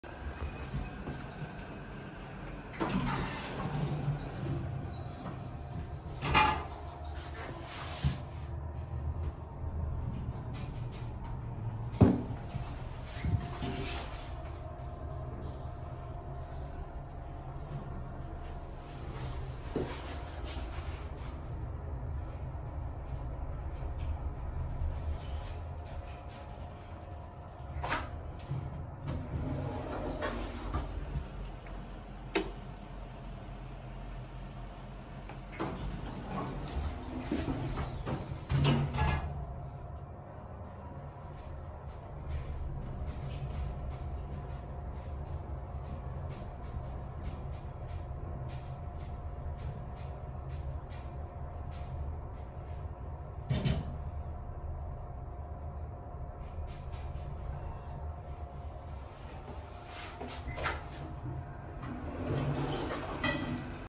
TOSHIBA CV60 MR @ HOTEL 81 JOY
Hotel 81 is an economy hotel chain which caters mainly to the budget hotel sector, although they do have slightly upmarket properties known as 'Hotel 81 Premier'. While some of their properties are directly owned by the company, others may been operated by franchisees such as 'Green City Holdings'.

This hotel, the Hotel 81 Joy, is not operated by the company. Instead, it is operated by a franchisee that goes by the name of 'Green City Holdings' as seen on their lift's Permit to Operate certificate. As a slight change of the usual pattern of franchisee hotels being older, the passenger lifts had one of their button panel replaced while leaving the other original panel intact. Otherwise, the overall condition of the hotel and the lift will give guests the feeling of checking into a hotel in the past.